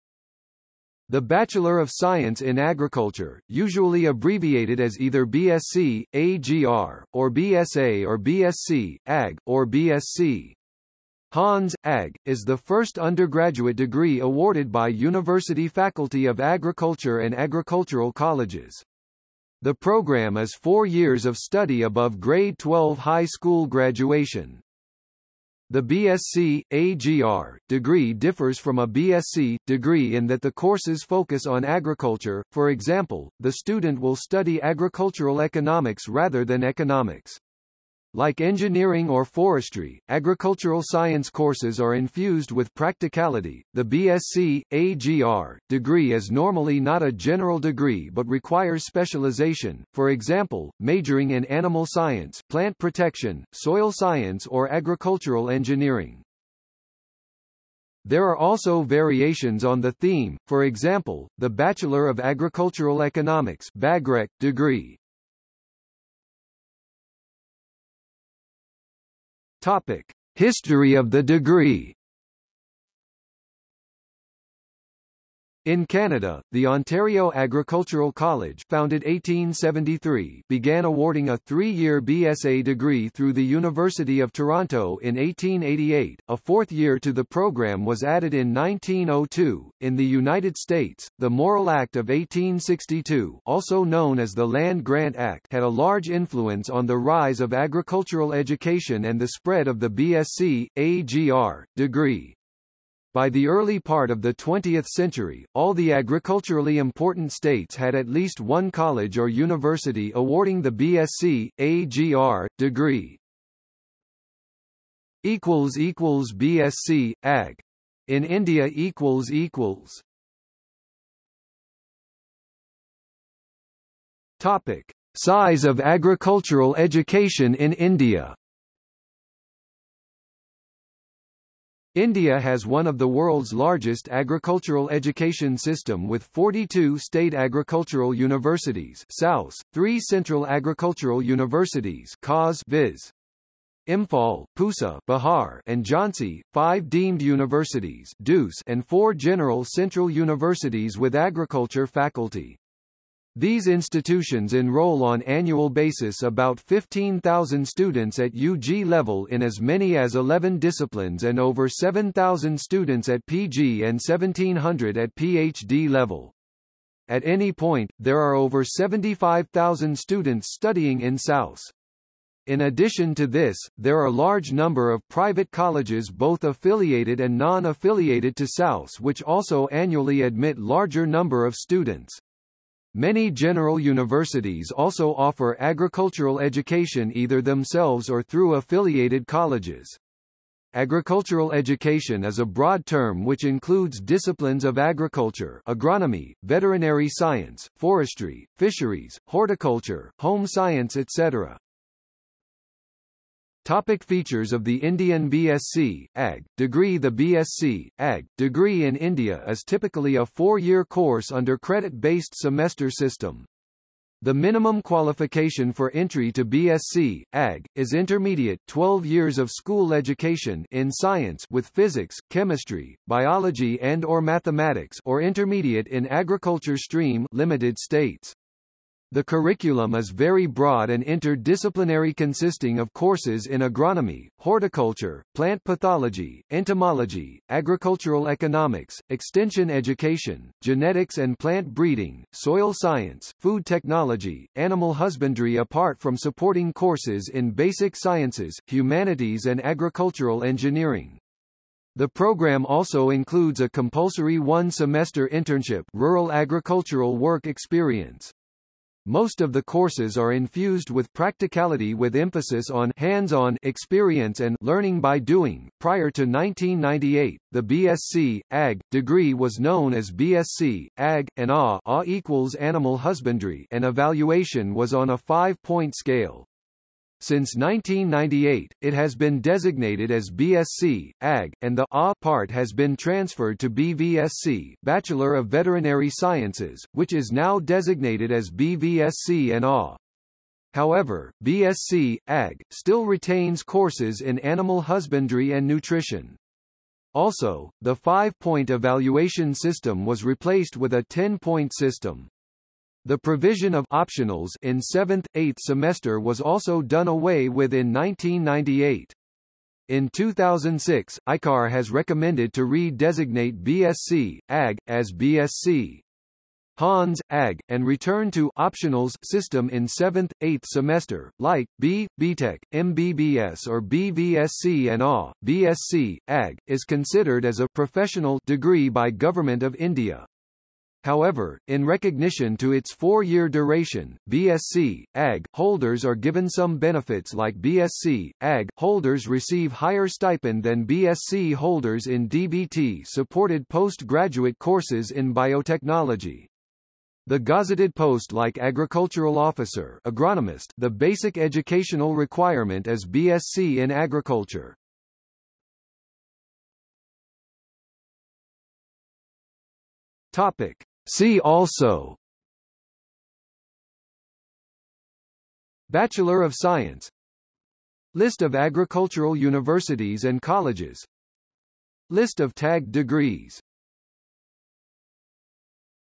Bachelor of Science in Agriculture | Wikipedia audio article
00:01:06 1 History of the degree
00:01:55 2 B.Sc.(Ag.) in India
00:02:01 2.1 Size of agricultural education in India
00:03:23 2.2 Features of the Indian B.Sc.(Ag.) degree
00:05:56 3 See also

- Socrates

SUMMARY
The Bachelor of Science in Agriculture, usually abbreviated as either B.Sc.(Agr.) or B.S.A. or B.Sc.(Ag.) or  B.Sc. (Hons.) Ag. is the first undergraduate degree awarded by university faculty of agriculture and agricultural colleges. The program is 4 years of study above Grade 12 High School graduation.

The B.Sc.(Agr.) degree differs from a B.Sc. degree in that the courses focus on agriculture: for example, the student will study agricultural economics rather than economics. Like engineering or forestry, agricultural science courses are infused with practicality.The B.Sc.(Agr.) degree is normally not a general degree but requires specialization: for example, majoring in animal science, plant protection, soil science or agricultural engineering.
There are also variations on the theme: for example, the Bachelor of Agricultural Economics (BAgrEc) degree